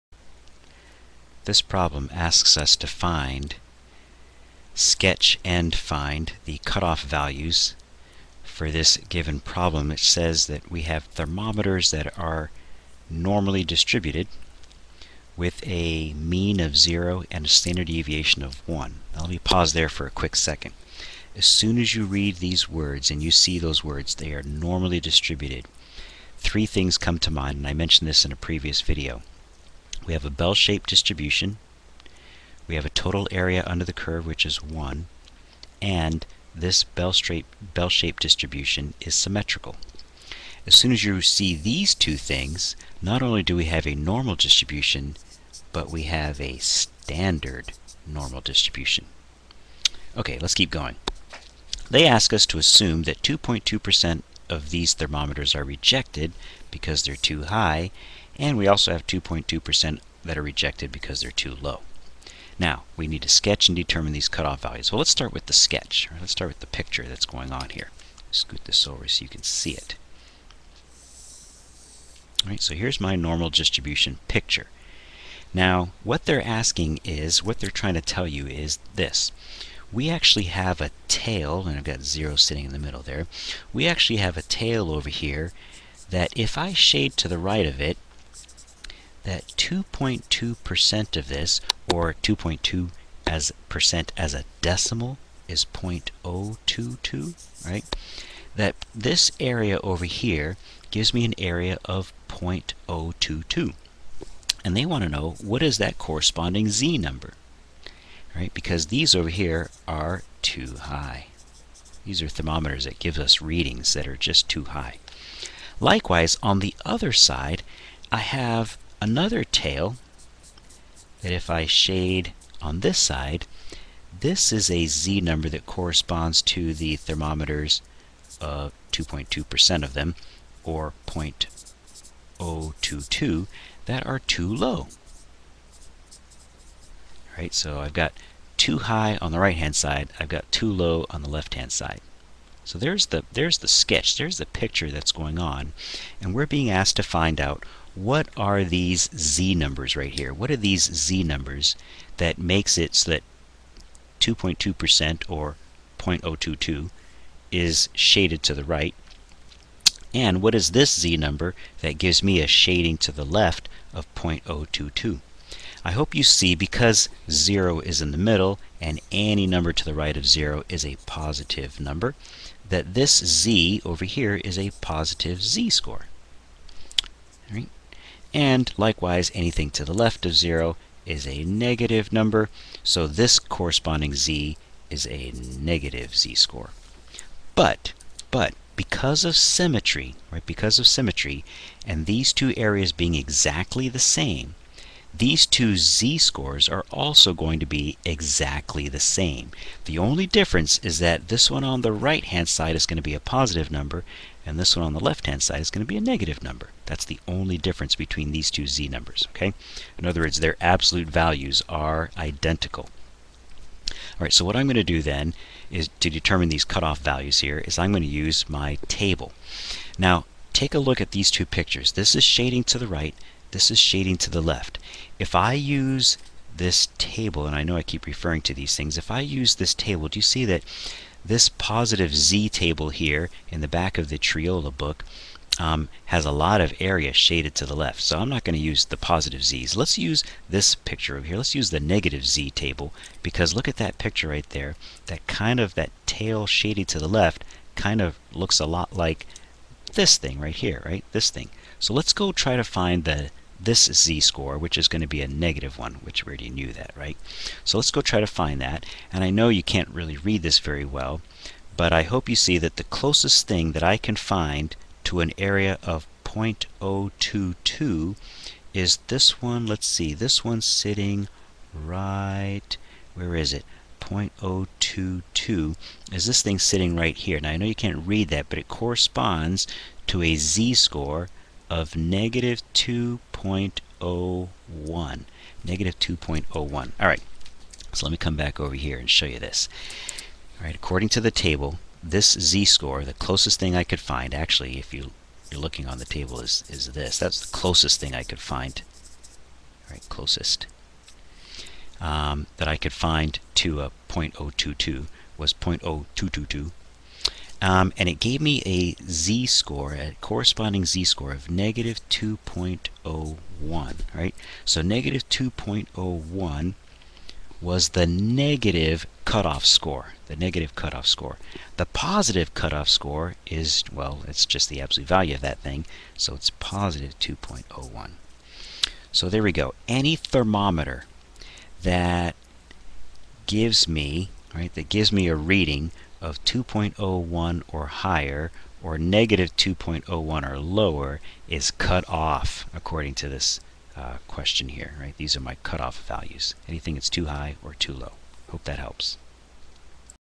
Stats- Finding Cut-off Values given Standard Normal Distribution - poyser.mp4
Demonstrates how to find cutoff scores (z-scores) that correspond to a given %, assuming a standard normal distribution.